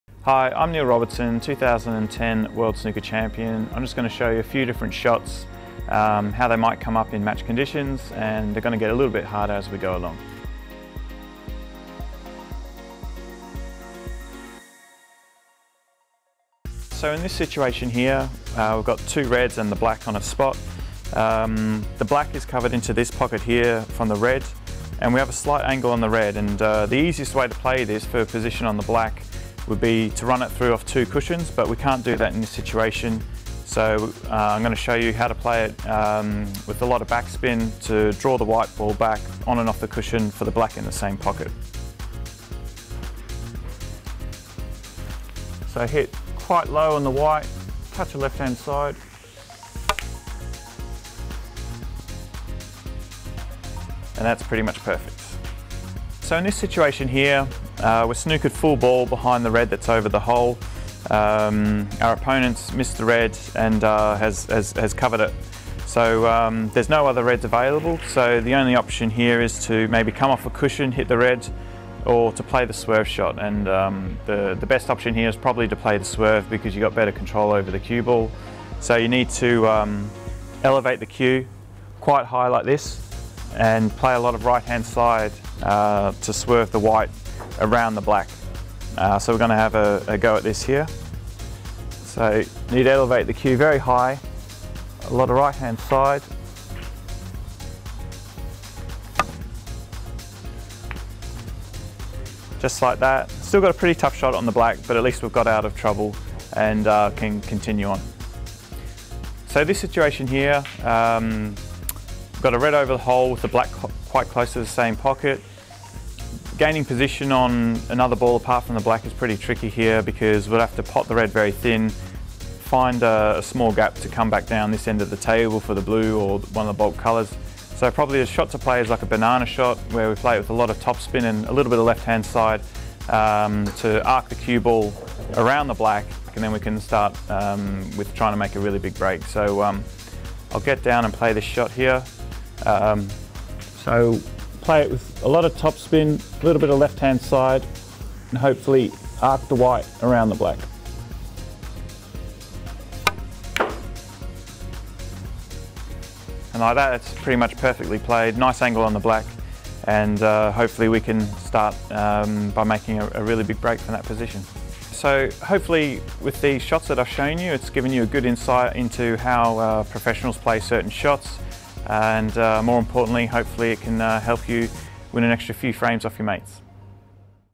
Neil Robertson's Snooker Masterclass
Professional Snooker player Neil Robertson gives a snooker masterclass.

Part of my graduation project for the National Film & Television School.

© National Film & Television School 2018